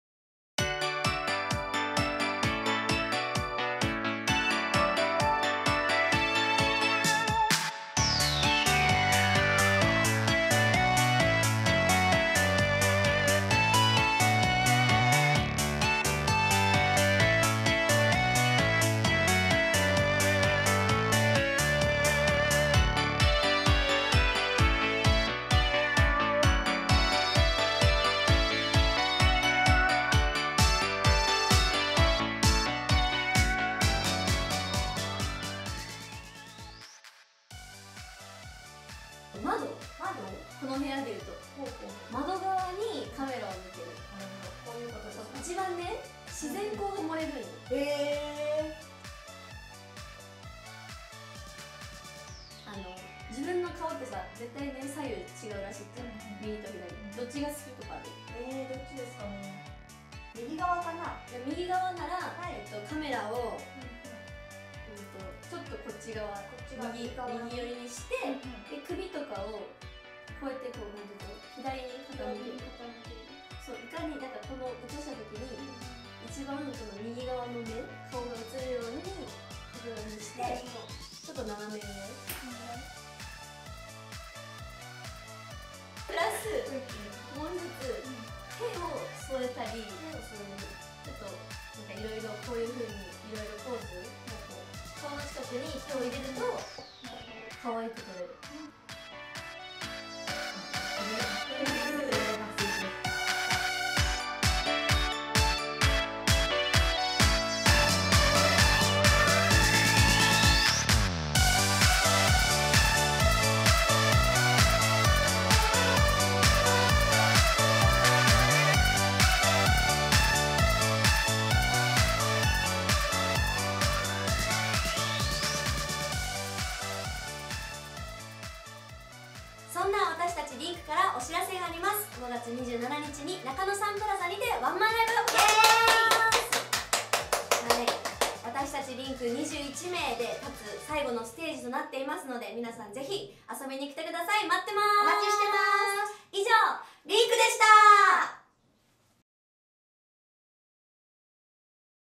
自撮り得意アイドルがマル秘テクを伝授
自撮りが苦手な後輩のために、LinQの新木さくらが立ち上がった！ 自身のSNSでかわいすぎる自撮りの数々を投稿する彼女が、うまく撮影するポイントを伝授。はたしてその効果のほどはいかに？